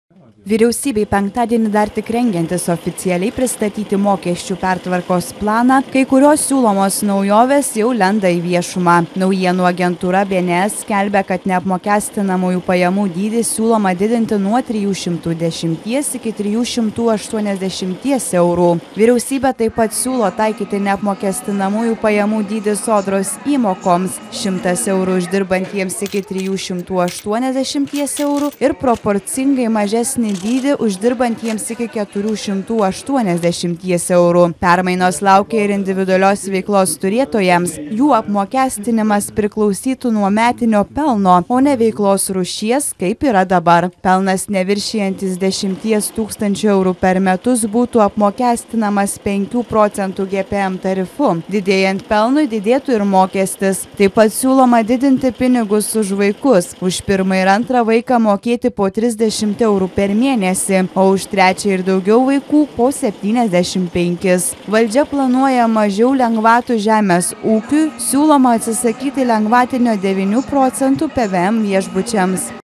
Aiškėja mokesčių pertvarkos detalės
Vyriausybei penktadienį dar tik rengiantis oficialiai pristatyti mokesčių pertvarkos planą, kai kurios siūlomos naujovės jau lenda į viešumą. Naujienų agentūra BNS skelbia, kad neapmokestinamųjų pajamų dydį siūloma didinti nuo 310 iki 380 eurų.